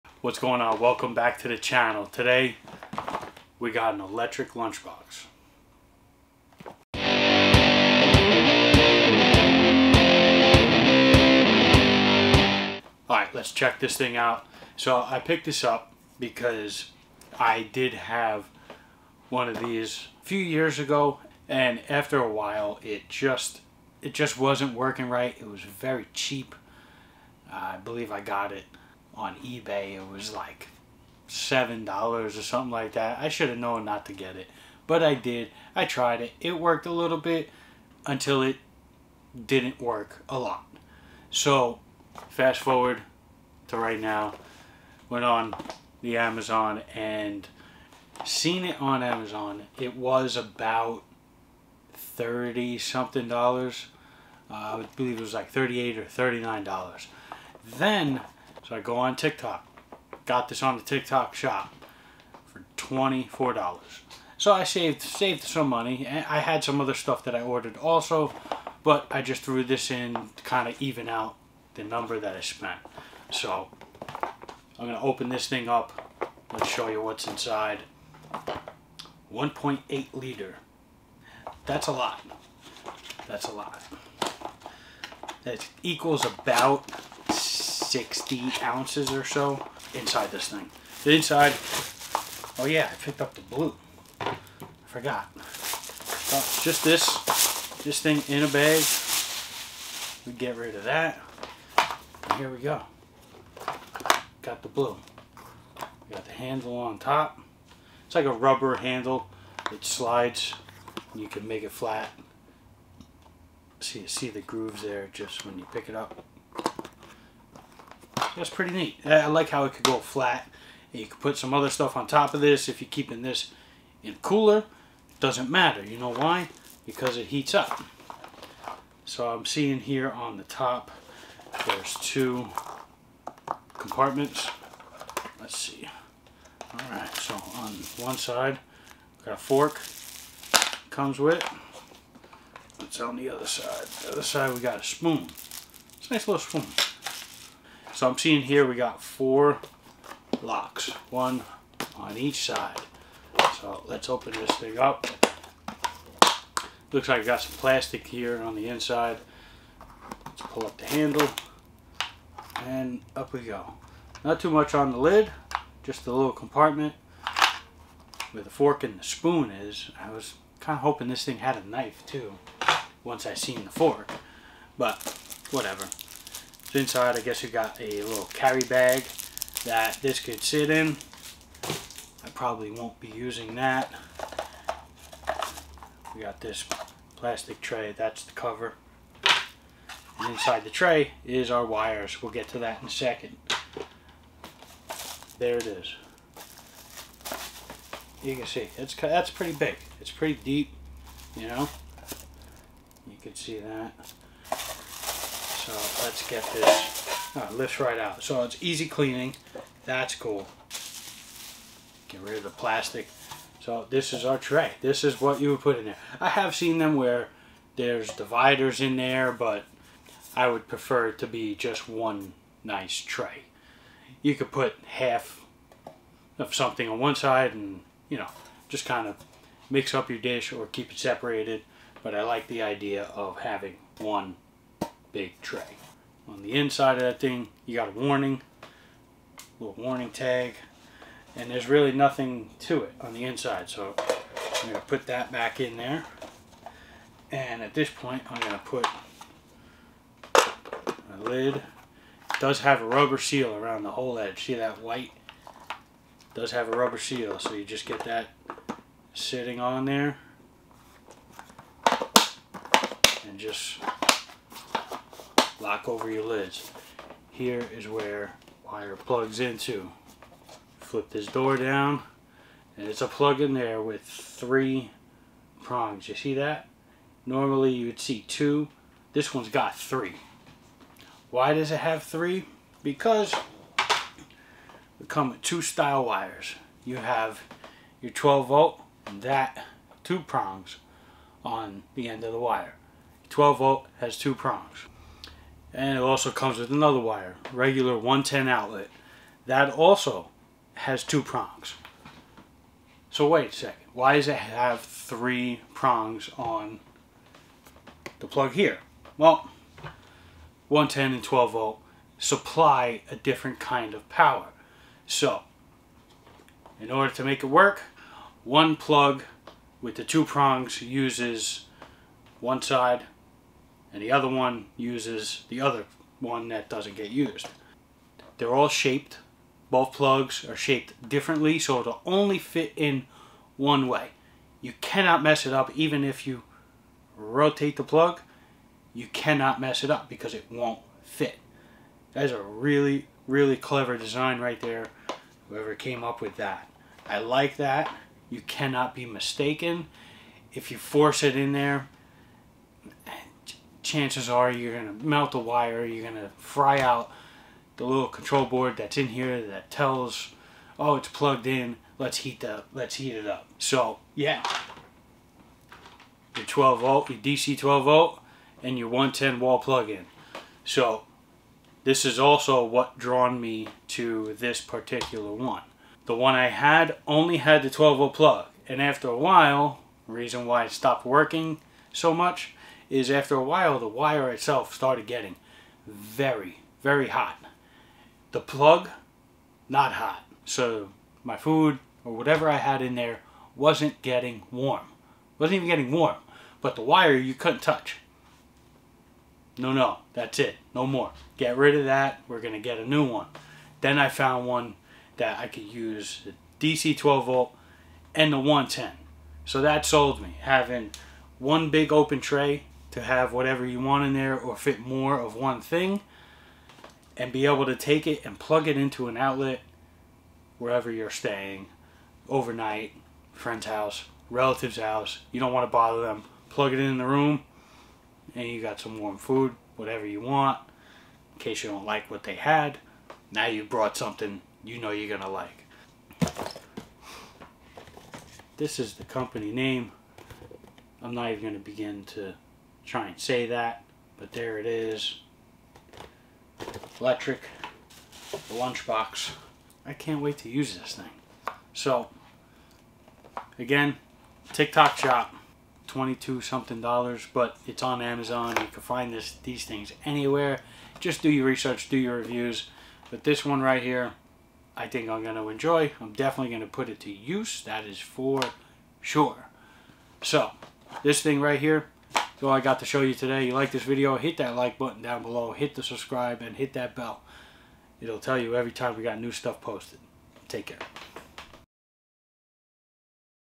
Electric Lunchbox Whats This ??????????
This is perfect for having hot lunches from home check here how it works and why you need one